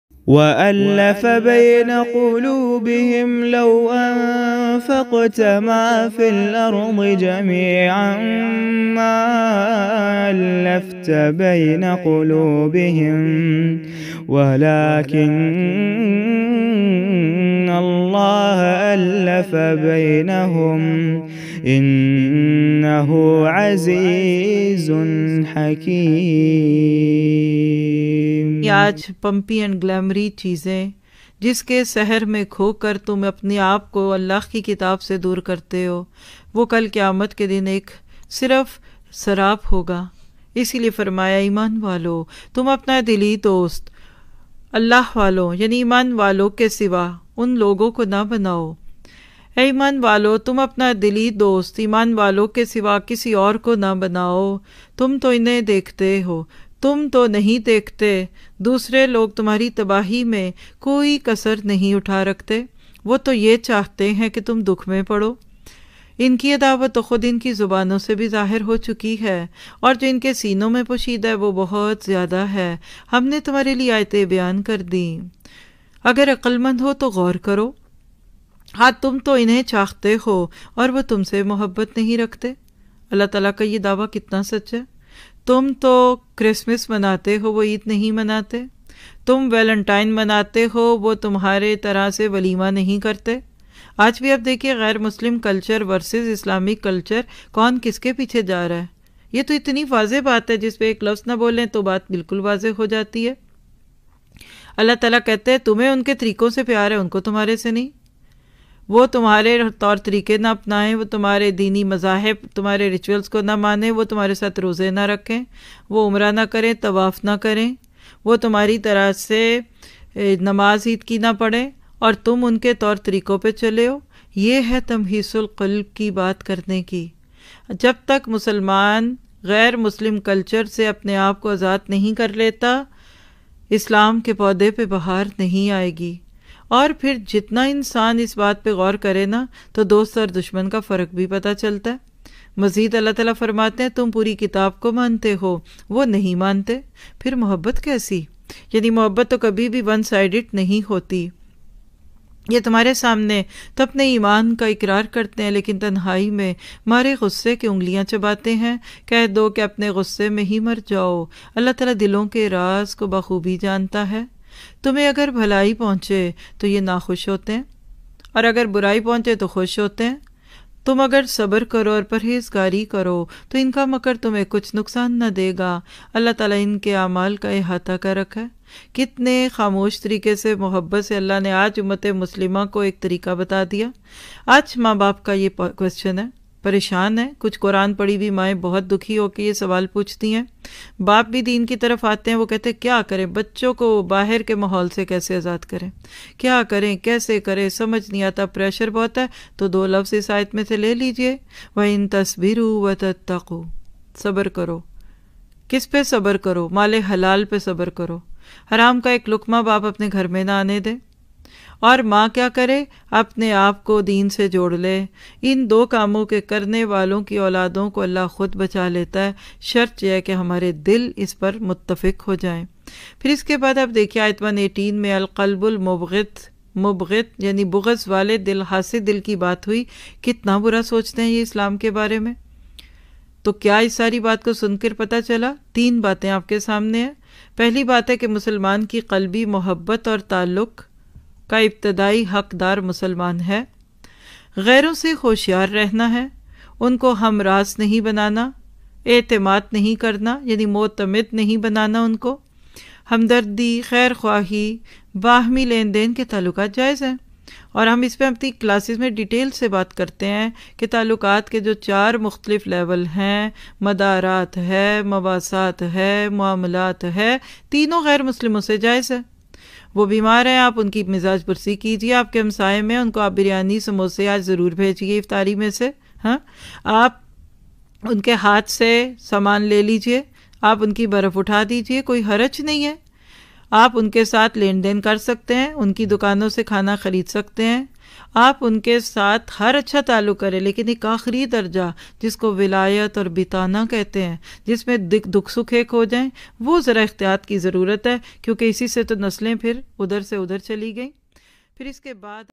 Quranic Gems |Ghair Muslim Se Dosti Ya Dili Mohabbat Rakhna: Juz 4 | NurulQuran
ghair muslim se dosti ya dili mohabbat rakhna
غیر مسلم سے دوستی یا دلی محبت رکھنا

Language : Urdu

For the complete Lecture of this Video visit:👇🏻

Dawrah E Quran 2021 : 👇🏻

Nurulquran.com
Nurulquranlive.com